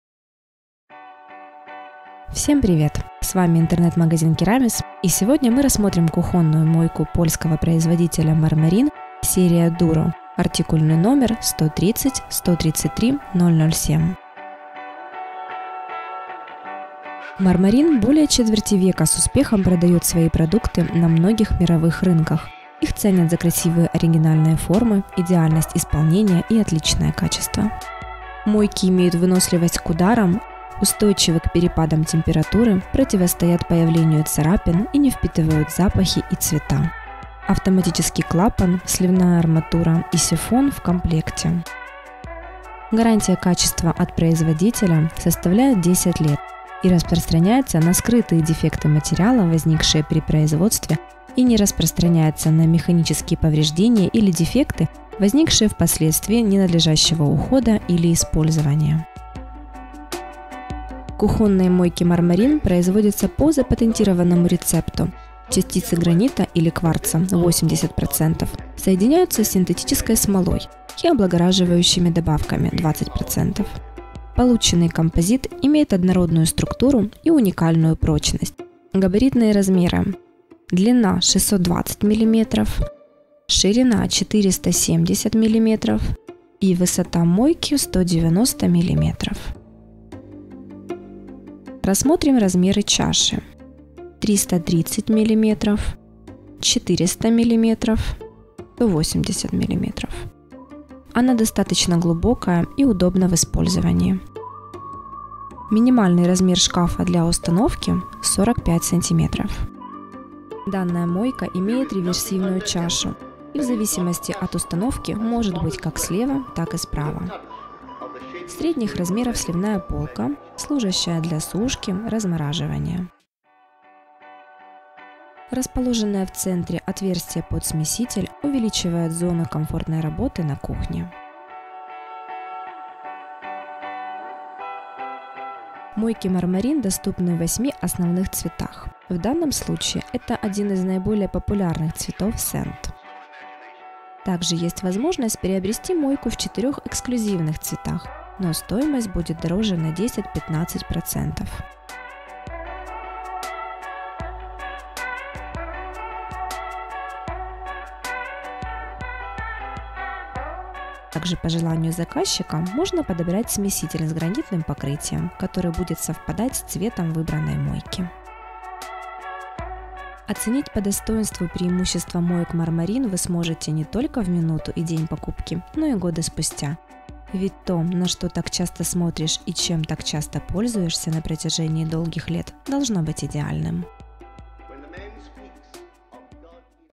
Мойка Marmorin Duro sand (130133007)
Обзор кухонной мойки Marmorin Duro sand (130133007)

В этом видеообзоре мы расскажем Вам о кухонной мойке Marmorin Duro (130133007) от польской компании Marmorin. Вы узнаете все о комплектации, характеристиках и особенностях данной модели.

Краткие характеристики:
Материал magranit
Размеры 620x470x190 мм
Размеры чаши 330x400x180 мм
Реверсивная чаша